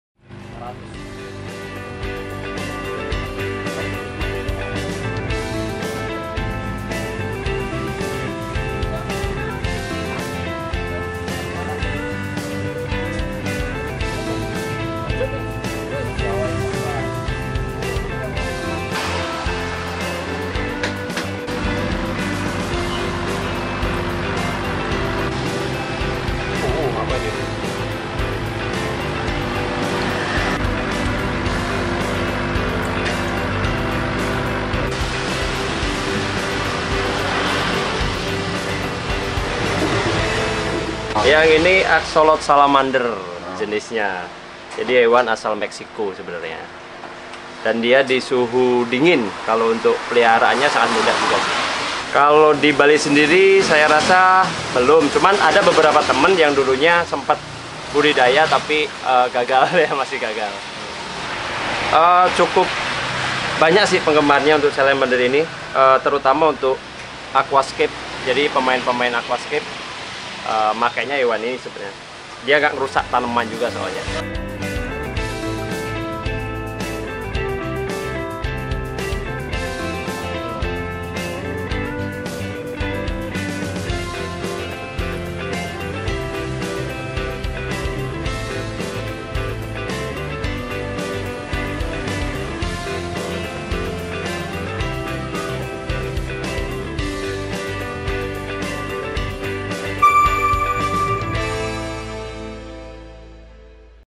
IKAN UNIK BERKAKI EMPAT MULAI DILIRIK PENGHOBI HEWAN
Salamander Axolotl menjadi perhatian penggemar hewan karena memiliki empat kaki yang menggantung saat berenang selain itu, bentuk insangnya unik seperti kipas yang menempel di kedua pipi. Keunikan itu membuat hewan amfibi ini mendapat julukan naga air, di Bali penggemarnya cukup banyak, namun belum ada yang bisa membudidayakan.